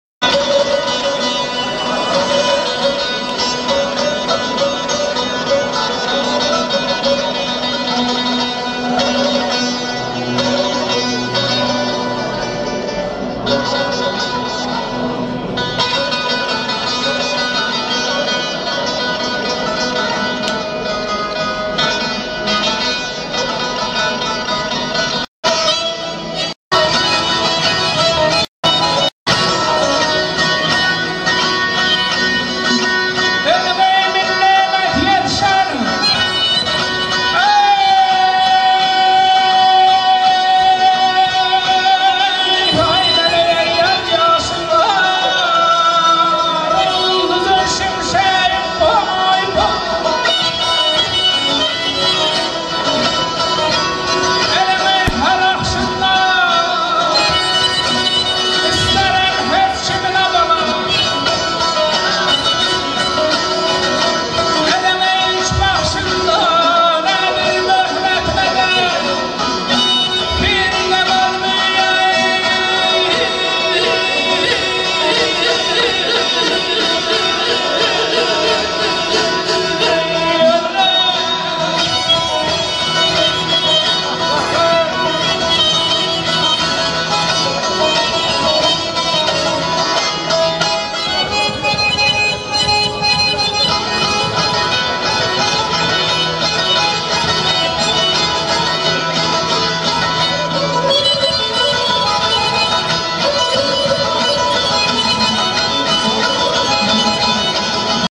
saz aşıq 2..Aşıq Nəbi, Aşıq Dəmir, Aşıq Nazim
"Ustad dərsi" aşıqlar məclisindən parçalar..